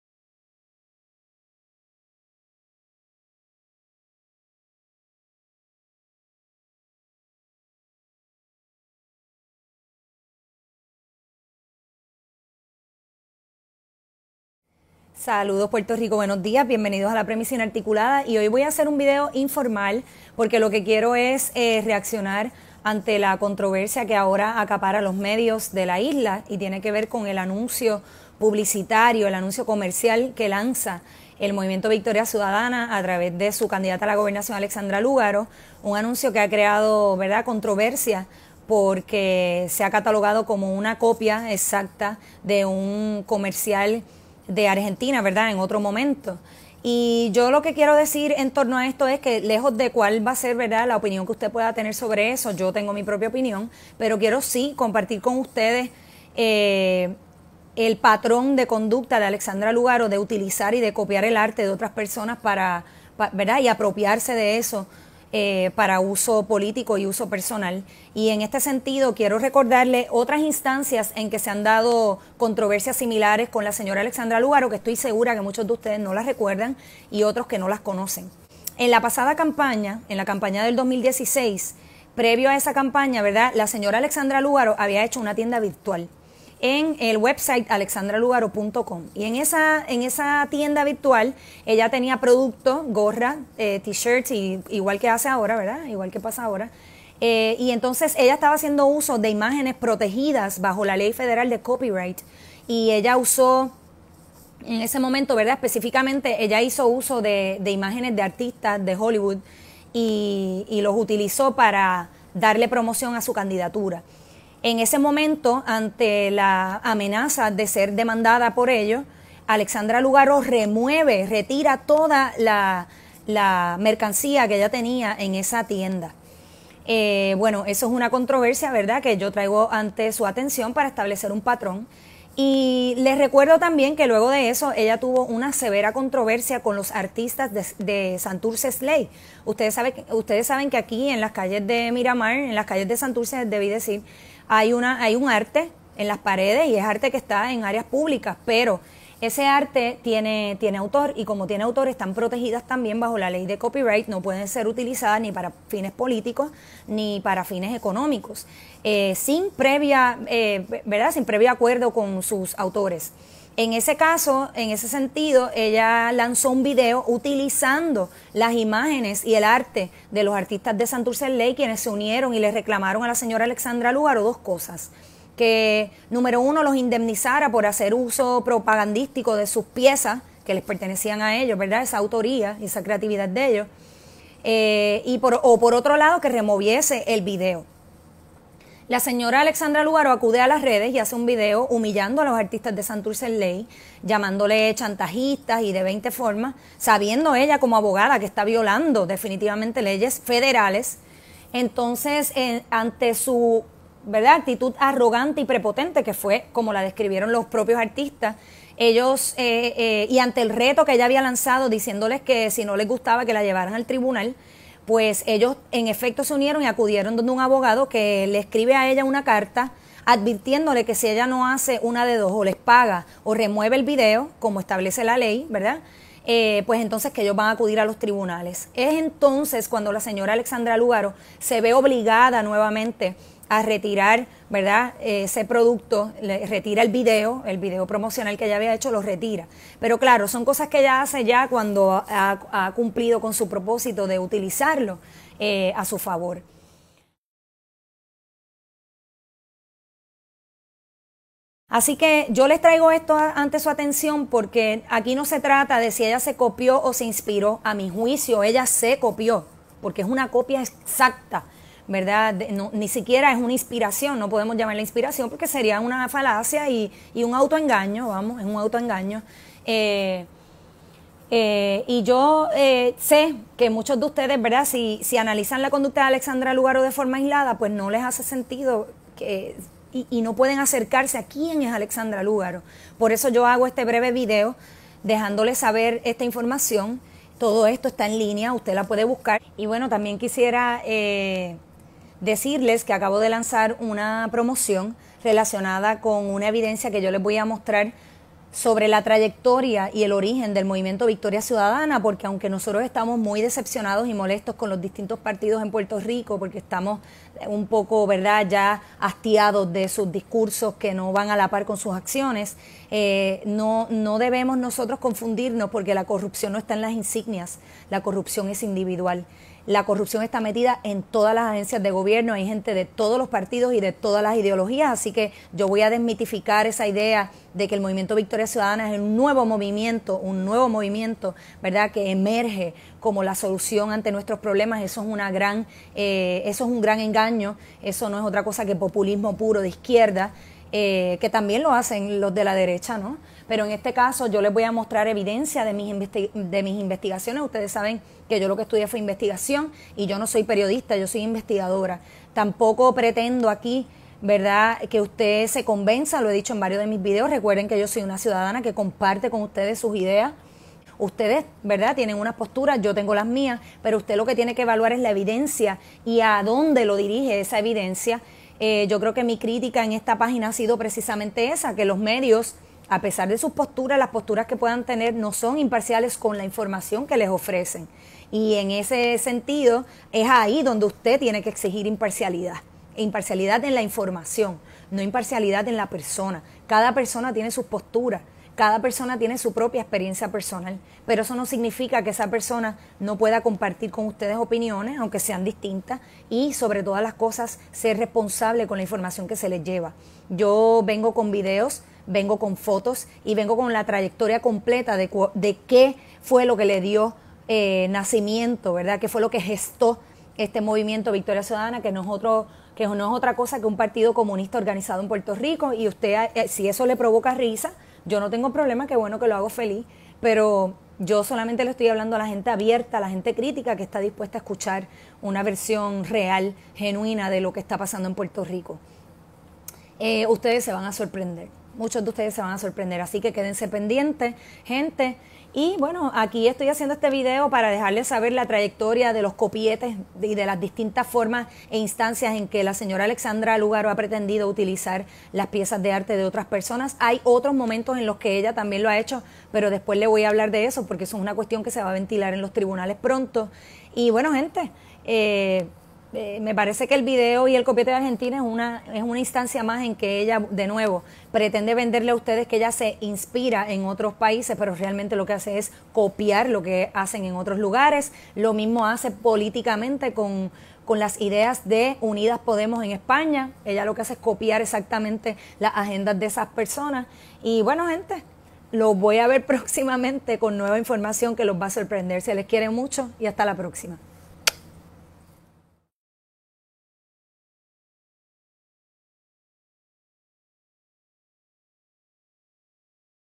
¿PLAGIO? EL VERGONZOSO PATRÓN DE DESHONESTIDAD CREATIVA DE ALEXANDRA LÚGARO.
¿SE COPIÓ ALEXANDRA LÚGARO, AGAIN?
Seamos francos, la honestidad intelectual y creativa no es una virtud que la Lcda. Lúgaro posee. No lo digo yo. Analicemos brevemente la EVIDENCIA sobre su trayectoria en torno a controversias previas relacionadas con violaciones a la ley federal de Copyright que probablemente muchos ya olvidaron o no conocen.

“Un candidato deshonesto, ejercerá un mandato deshonesto.”